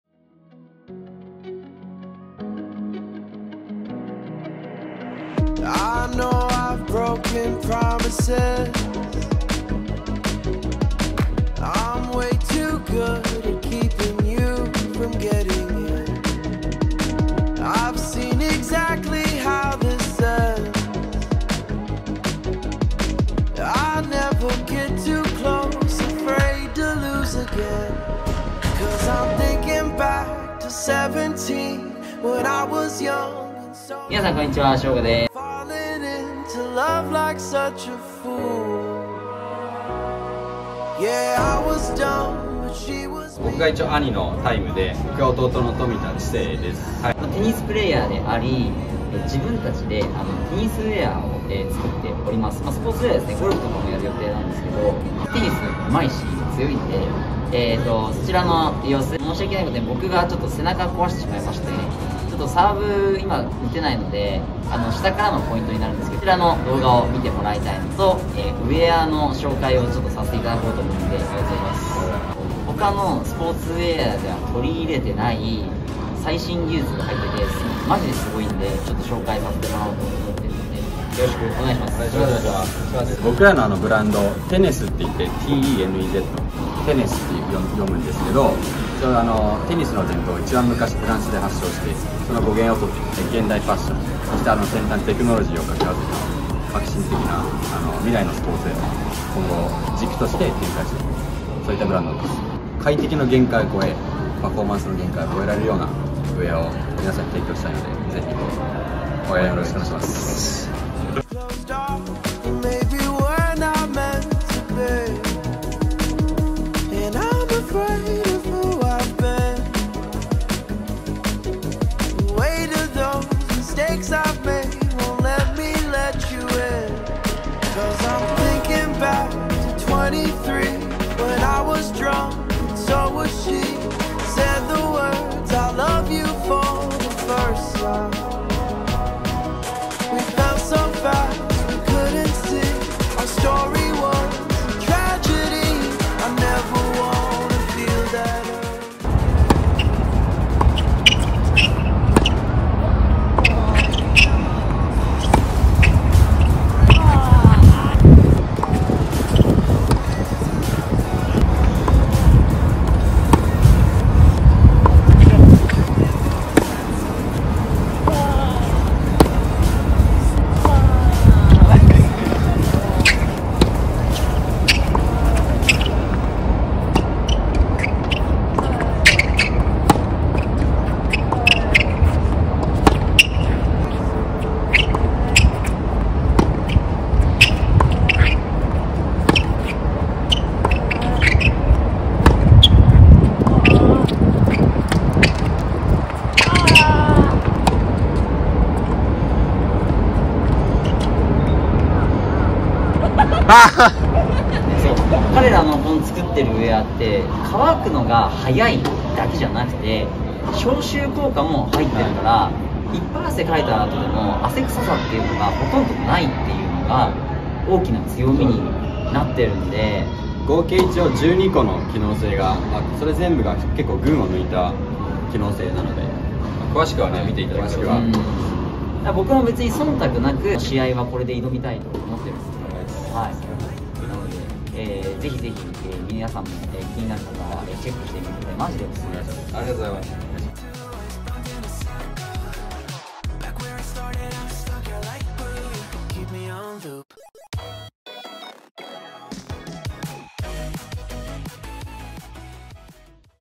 令和の虎で話題に！汗は一瞬で乾き、消臭効果もある最強のテニスウェアを作った双子とテニスしてみた
• 彼らの新プロジェクト、ウェアはこちらから

佐藤翔吾戦術動画公式ライン
フィジカル差を埋めるレッスン動画

佐藤翔吾公式ライン戦術電子書籍
SHOGO RUN CLUB
（エンドロールに名前入れたい方はこちら）